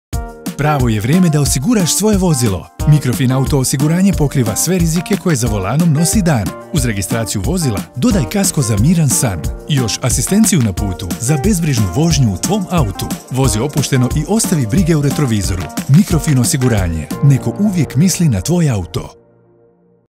MFO - Osiguranje motornih vozila
TV spot za Mikrofin Osiguranje
"Neko uvijek misli na tvoj auto"